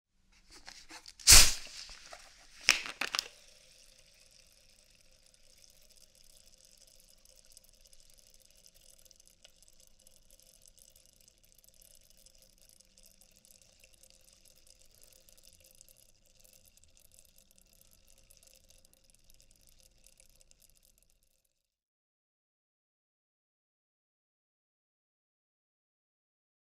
Plastic Bottle Cap Opening Sound Effect | Royalty-Free Sound Effects [Download]
Sparkling water bottle sound effect. This sound includes the opening of a carbonated water bottle. You'll the rotating of cap and fizzing sounds made by the sparkling water.

Cinela Albert